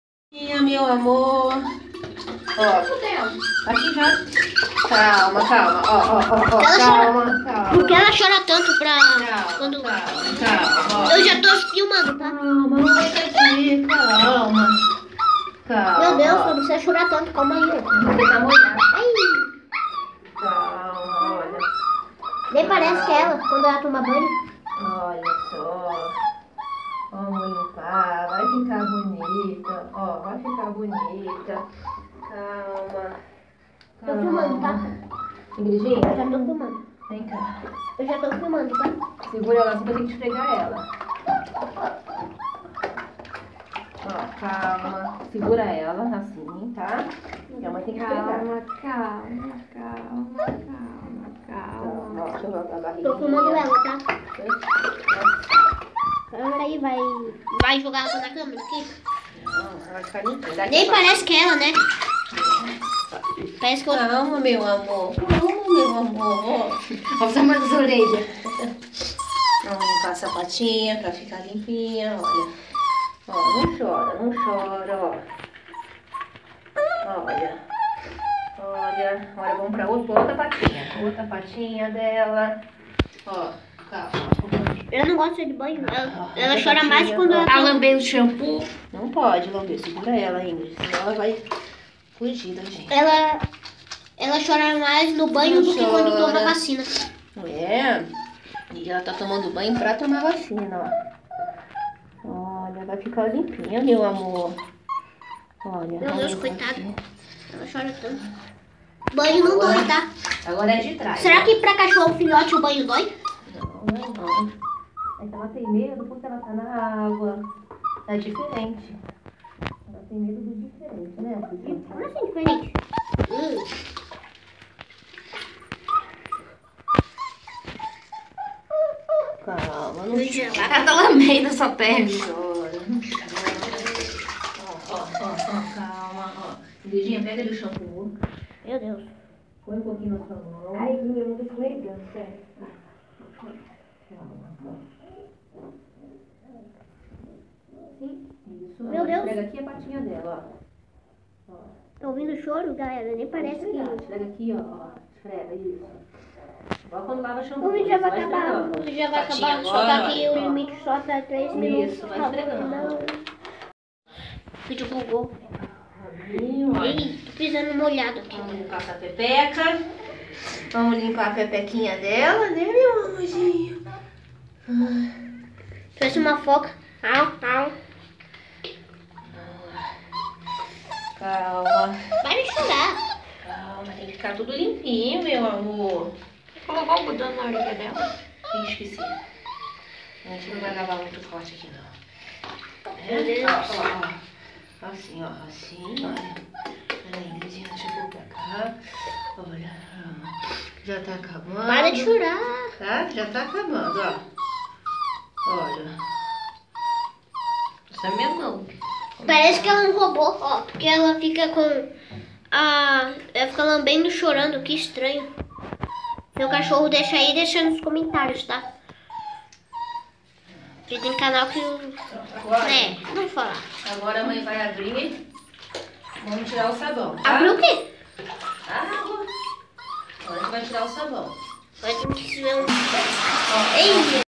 Myna tomando banho
Poodle com 3 meses tomando banho no chuveiro